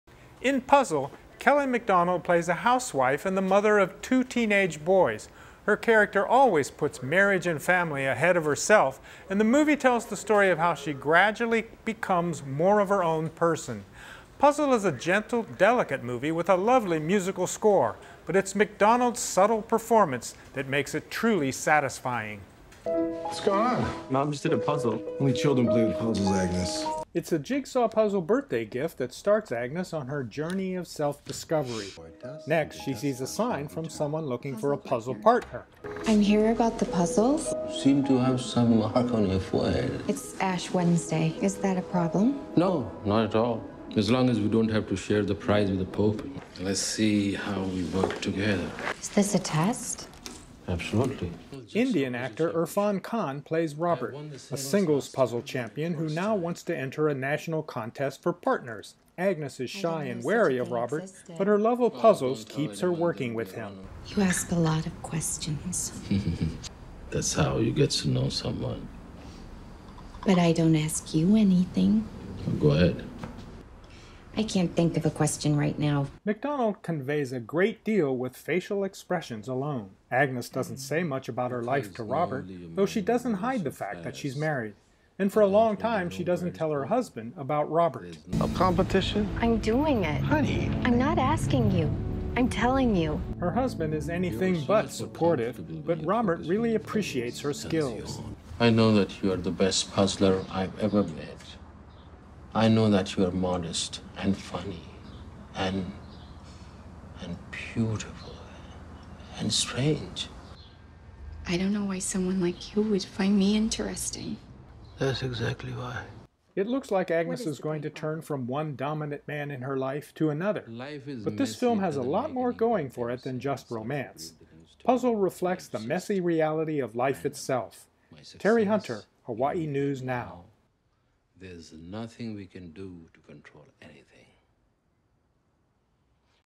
Terry Hunter reviews "PUZZLE"
The Scottish actress Kelly Macdonald gets her first starring role in a feature film called PUZZLE. In this film she plays a housewife and the mother of two teenage boys. Her character always puts marriage and family ahead of herself, and the movie tells the story of how she gradually becomes more of her own person. PUZZLE is a gentle, delicate movie with a lovely musical score. But it’s Macdonald’s subtle performance that makes it truly satisfying.